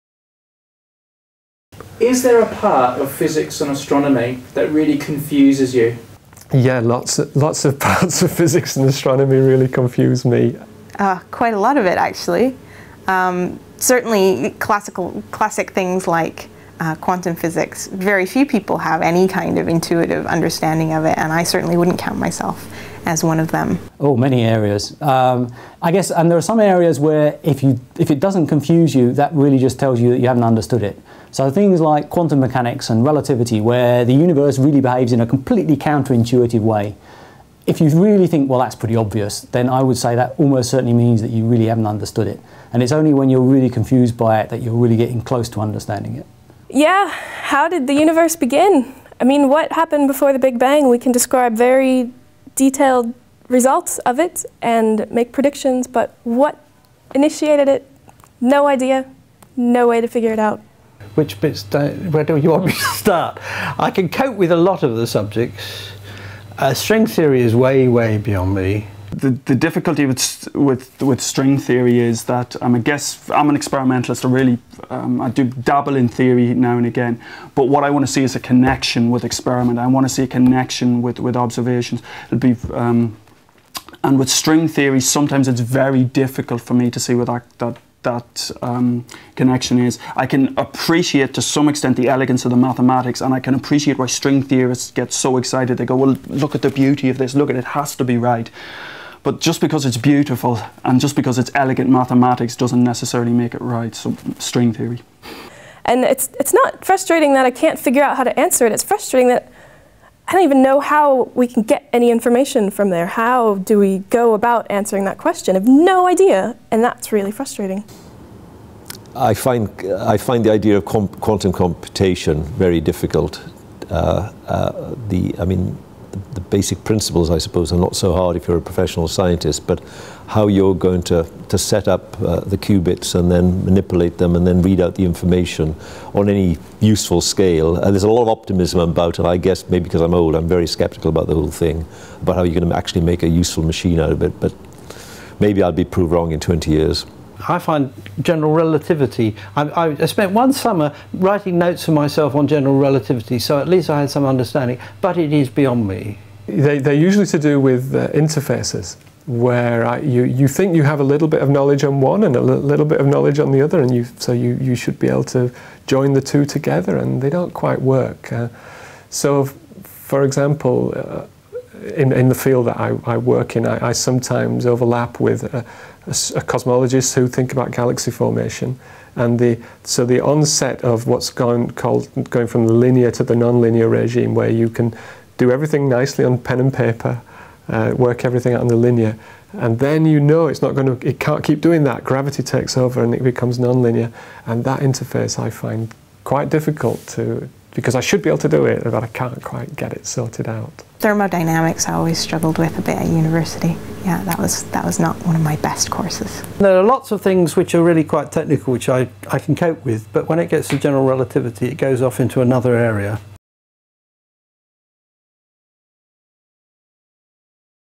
What confuses a physicist?
A viewer asks our team of physicists and astronomers what part of science they find most confusing?

With Ed Copeland and Meghan Gray and Mike Merrifield and Philip Morairty